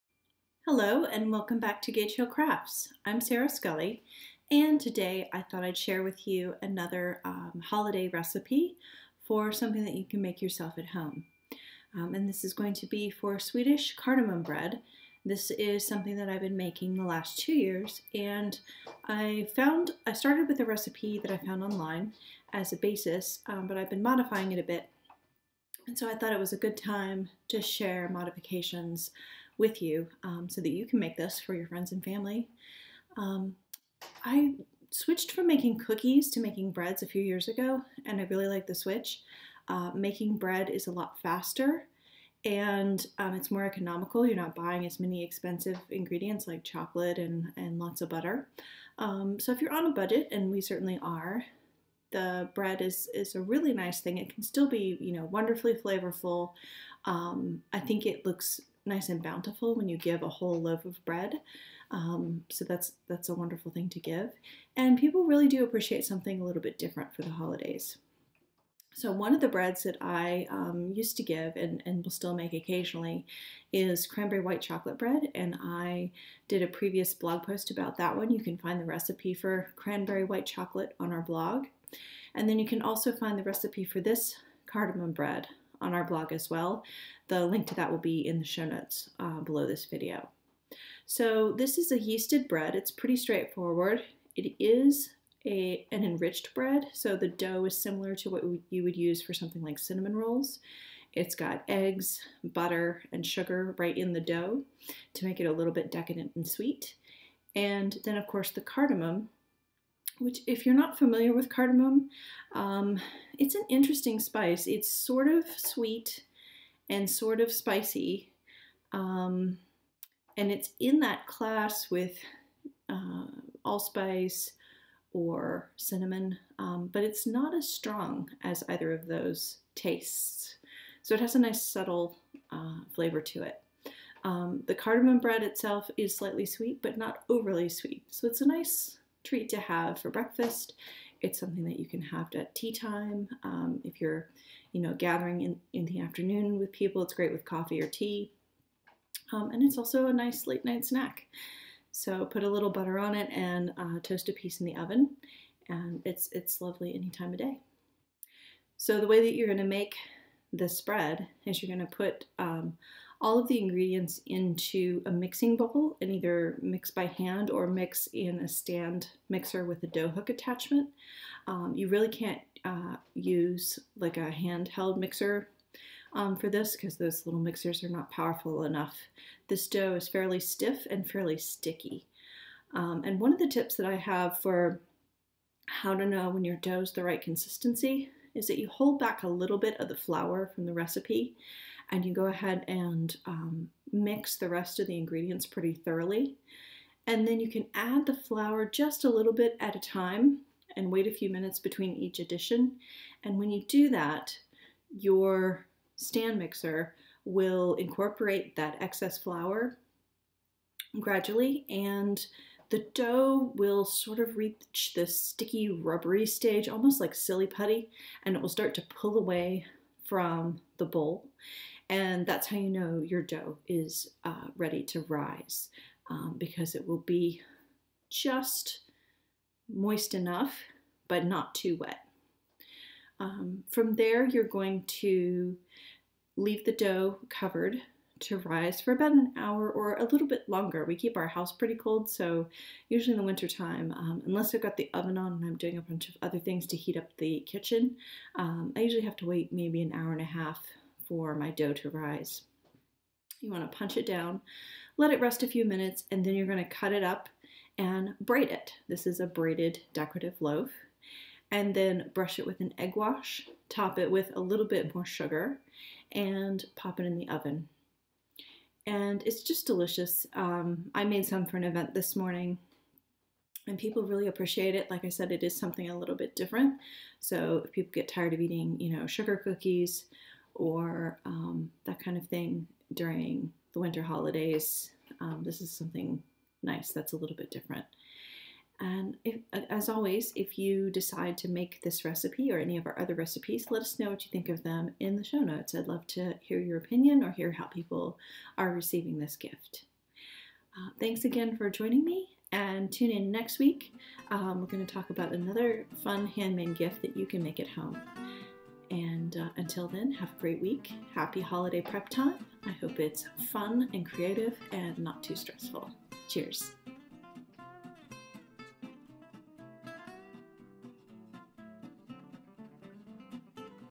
Sweedish Cardamom Bread: a delicious holiday treat
Sarah shares her modified version of a classic Sweedish bread, including a tip for knowing when your dough has just the right amount of moisture.